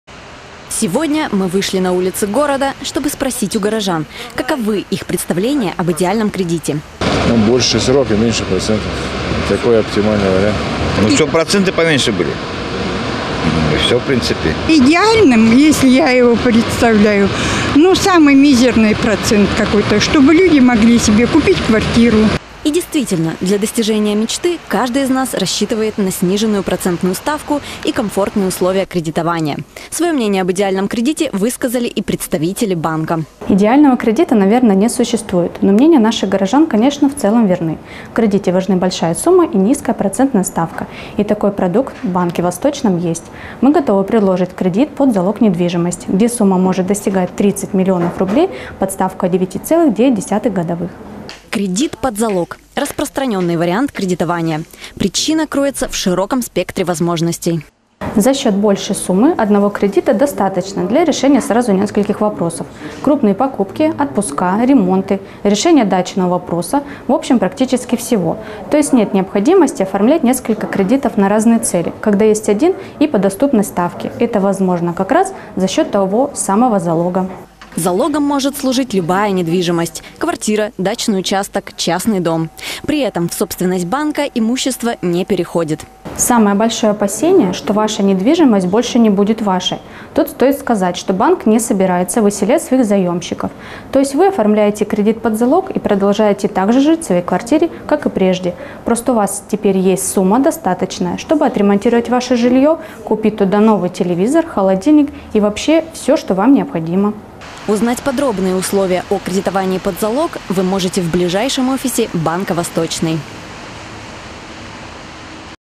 Оценить преимущества покупок в кредит под залог предлагает банк «Восточный»
Сегодня мы вышли на улицы города, чтобы спросить у горожан, каковы их представления об идеальном кредите. Отвечали так:

• «Больше сроки, меньше процентов. Такой идеальный вариант»;
• Чтоб проценты поменьше были. Ну и всё в принципе»;
• «Идеальным, если я его себе представляю, ну самый мизерный процент какой-то. Чтобы люди могли себе купить квартиру».

И действительно, для достижения мечты каждый из нас рассчитывает на сниженную процентную ставку и комфортные условия кредитования. Своё мнение об идеальном кредите высказали и представители банка.